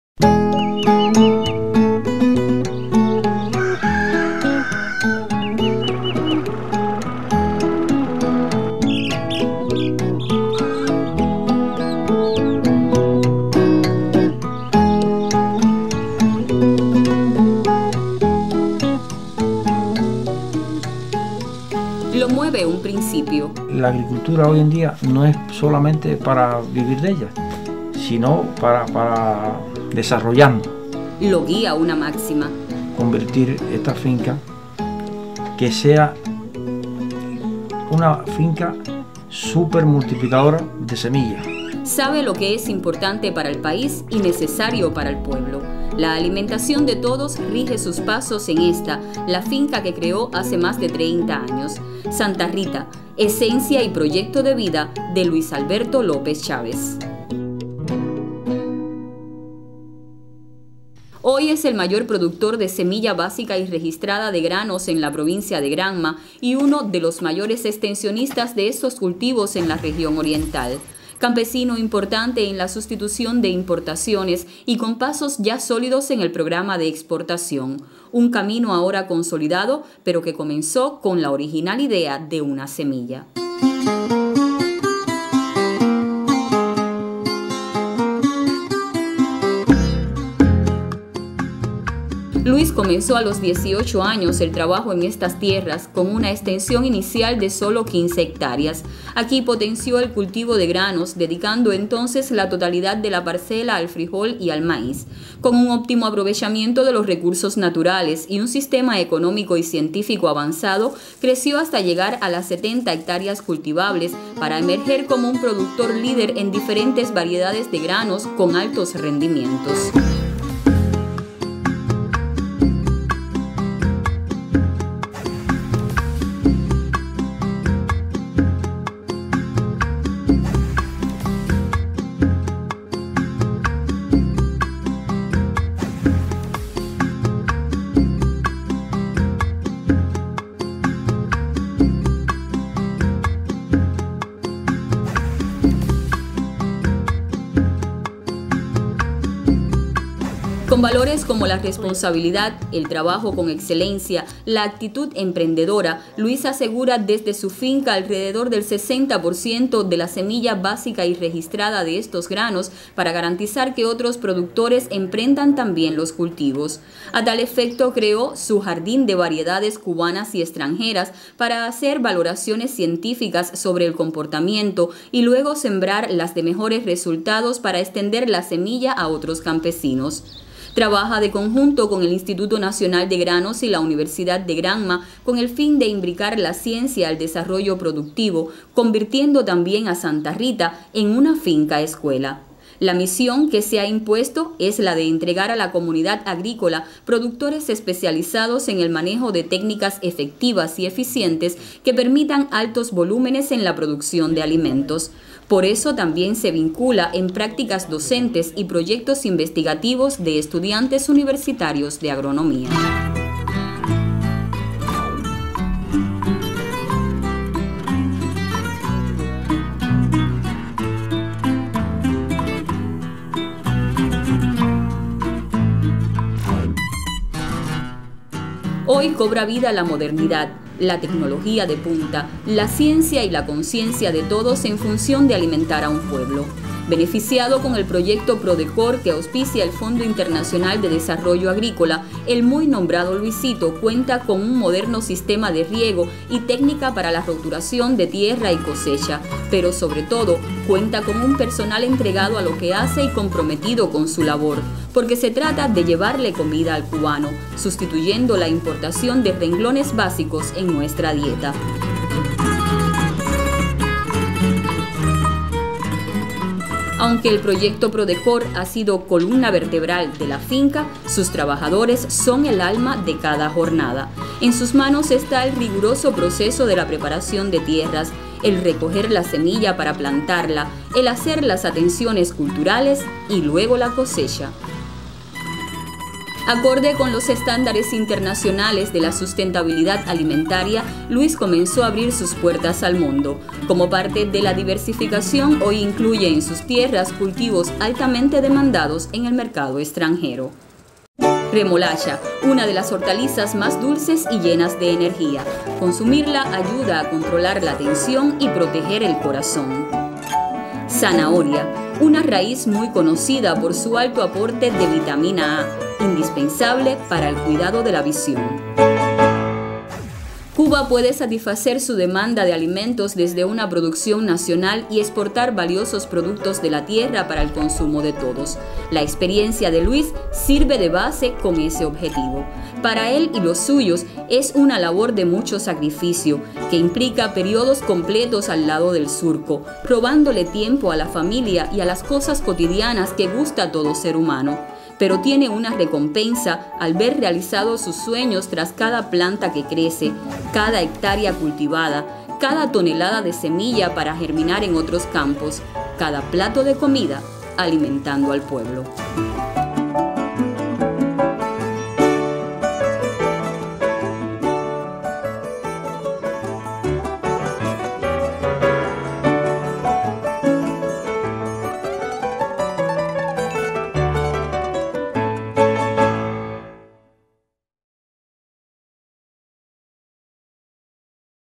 Santa Rita, el sueño realizado de Luis Alberto López Chávez
Desde la UNEAC en la Provincia Granma nos llega esta obra audiovisual que compartimos.